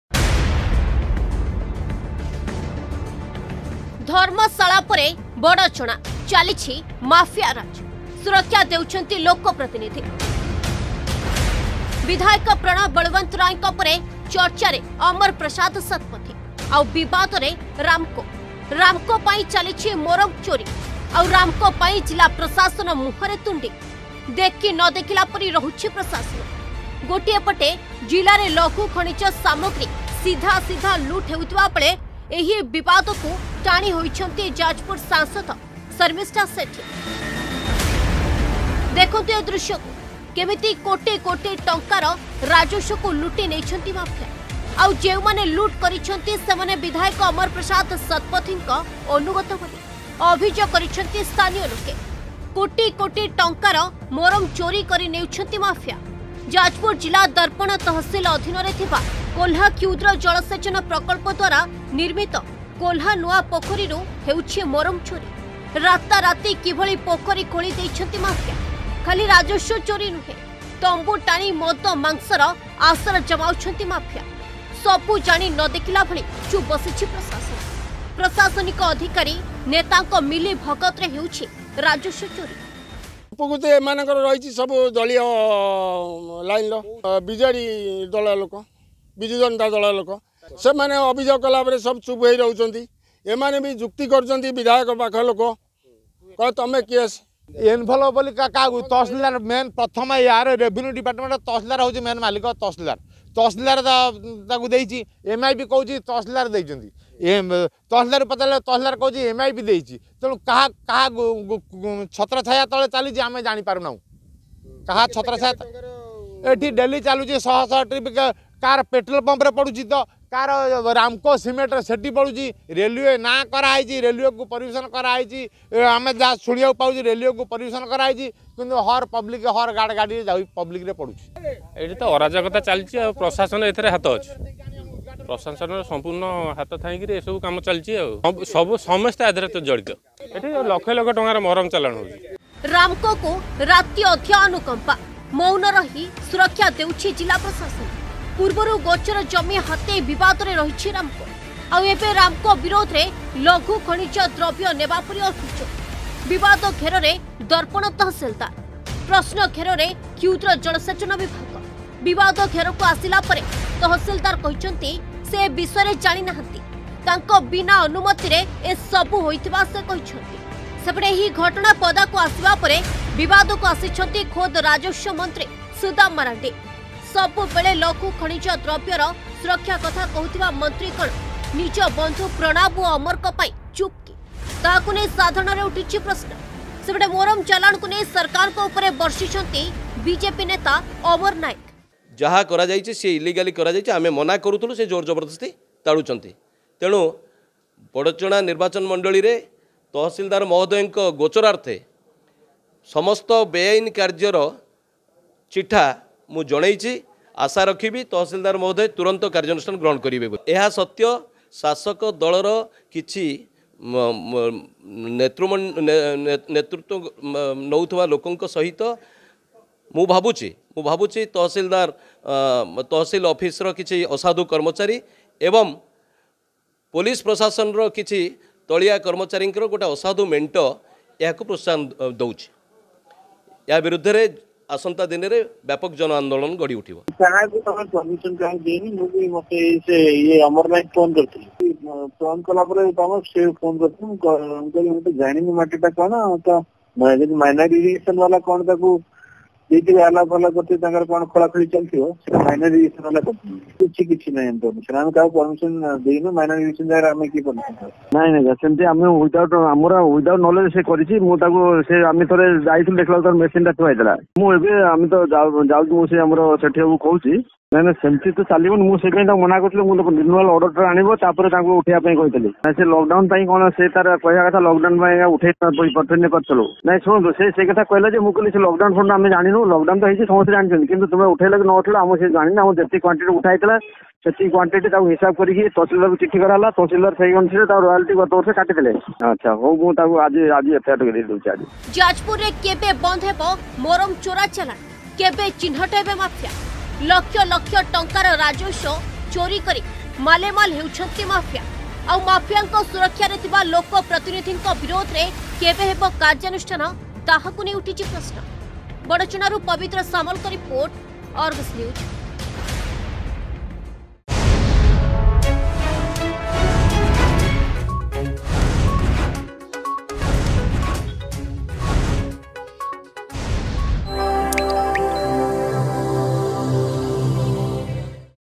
ଧର୍ମଶାଳା ପରେ ବଡଚଣା......ଚାଲିଛି ମାଫିଆ ରାଜ୍
ଧର୍ମଶାଳା ପରେ ବଡଚଣା...ମାତିଛନ୍ତି ମୋରମ୍ ମାଫିଆ. ଲୁଟ ଚାଲିଛି ଲଘୁ ଖଣିଜ ପଦାର୍ଥ. ବିଧାୟକ ଓ ସାଂସଦଙ୍କ ଛତ୍ରଛାୟା ତଳେ ରହି ମାଫିଆ ମାନେ କରୁଛନ୍ତି ରାଜସ୍ୱ ଲୁଟ. ଆଉ ଚେଇଁ ଶୋଇ ଦେଖୁଛି ଜିଲ୍ଲାପ୍ରଶାସନ...ନଜର ପକାନ୍ତୁ ଏହି ରିପୋର୍ଟ ଉପରେ
ଅଧିକ ଖବର ପାଇଁ ଡ଼ାଉନଲୋଡ କରନ୍ତୁ “Argus News” ଆପ୍